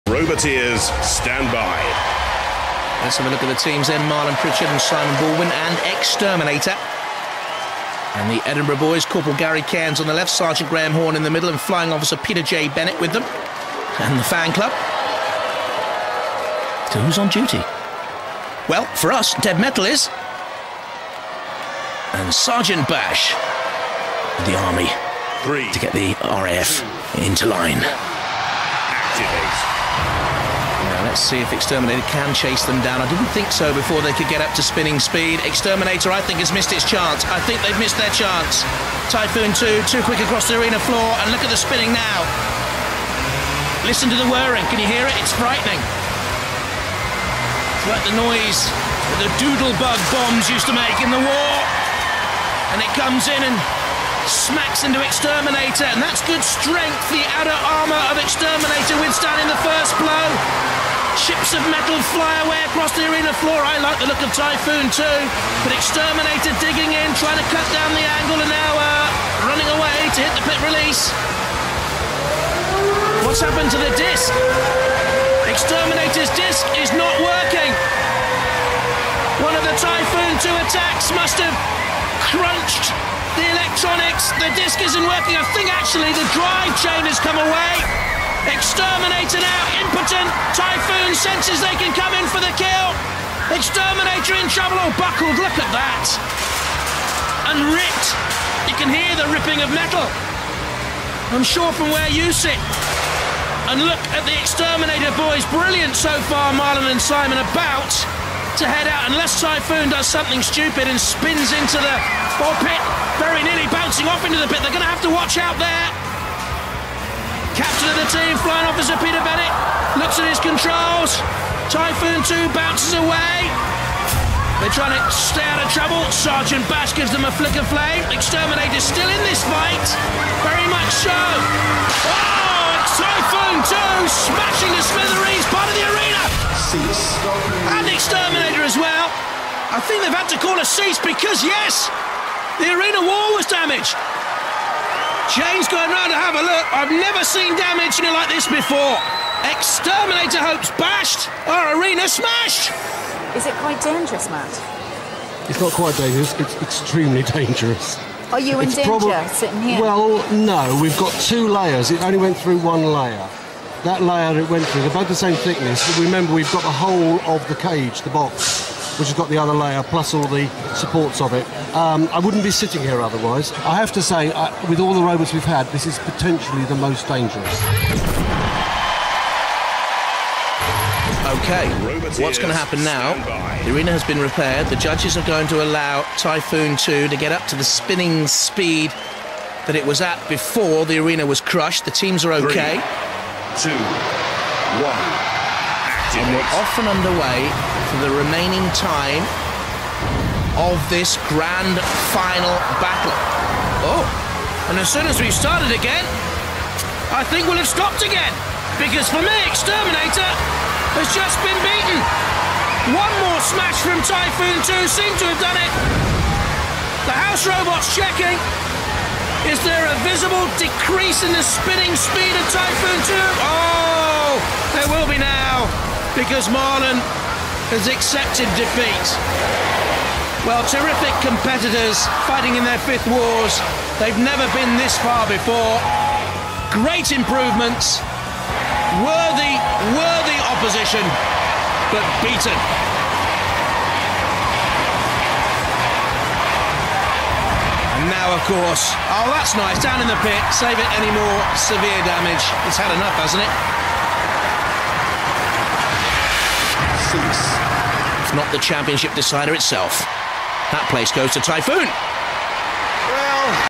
Crowned Classics Typhoon 2 vs X-Terminator
Two of the Seventh Wars' most powerful spinners clash in this deadly and destructive battle!